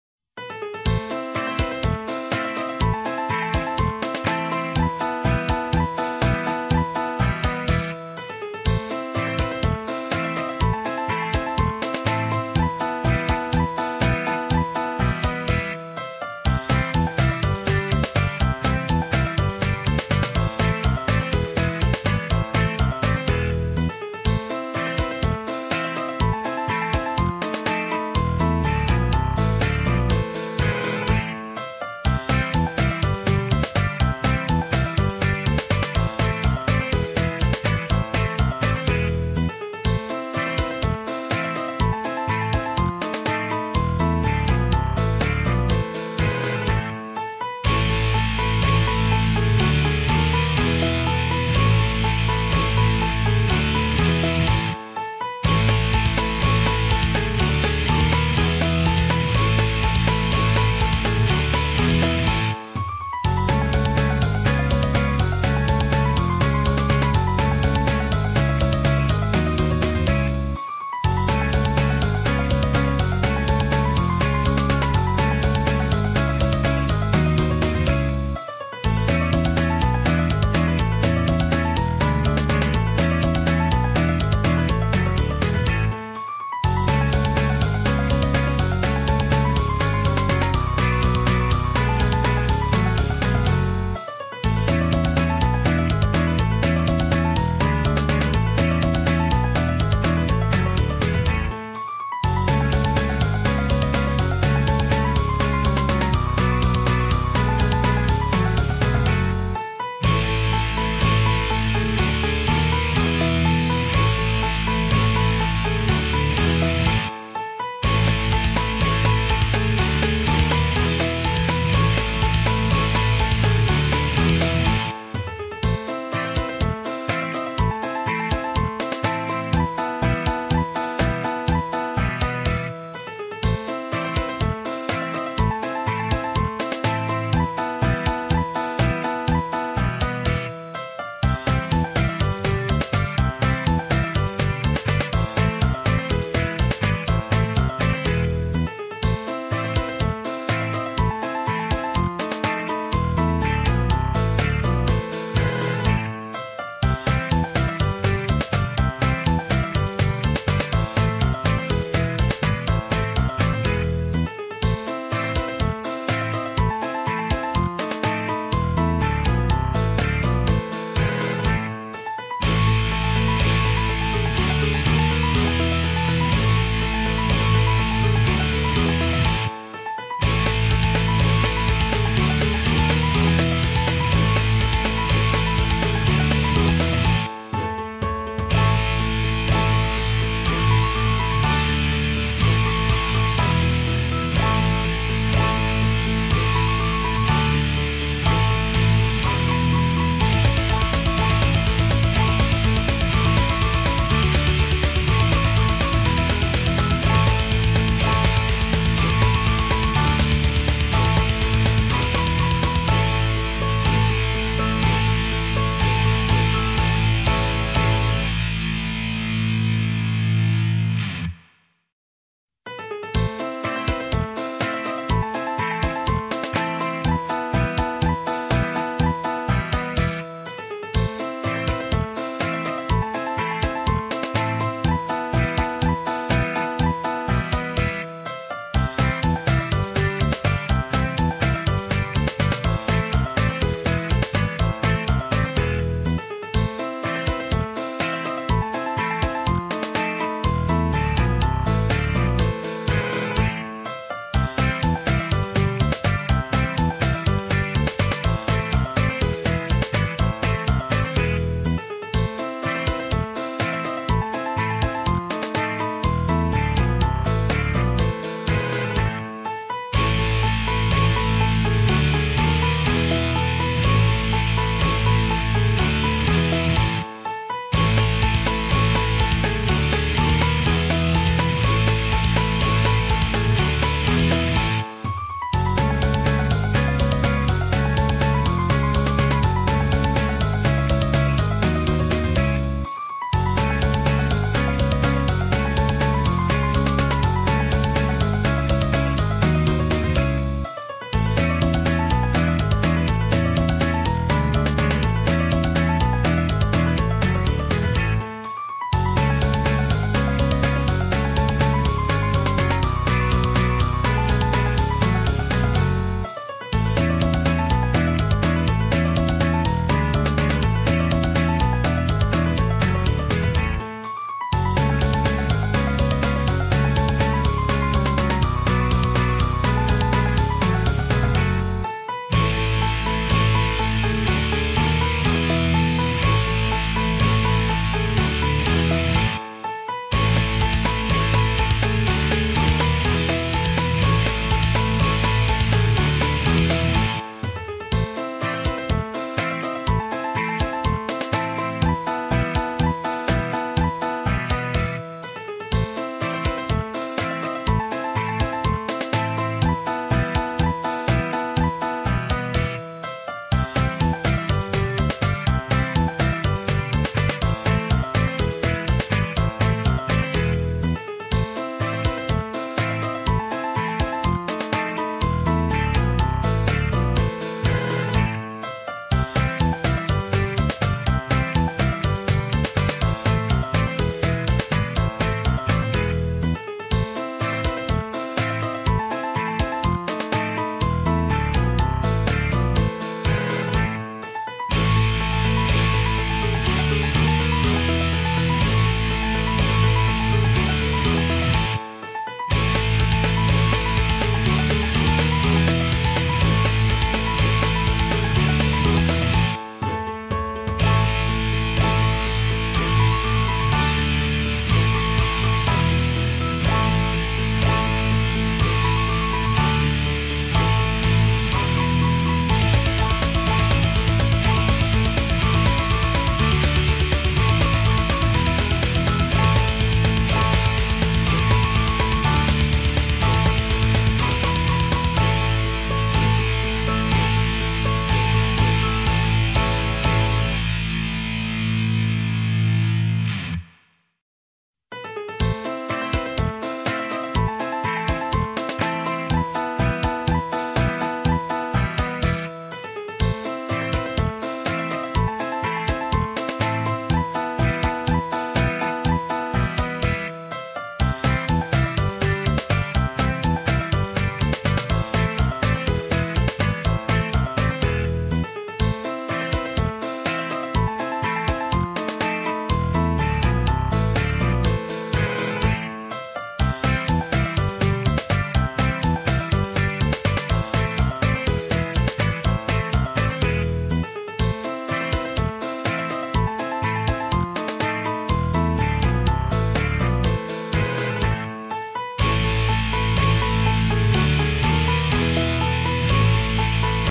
Turkish Dishes at Anadolu University 2013-2014.
Turkish dishes at Anadolu University supervised by the master
of Turkish Cusine there Osman Güldemir. All the photos of food dishes by Osman Güldemir and music by Daniel Bautista.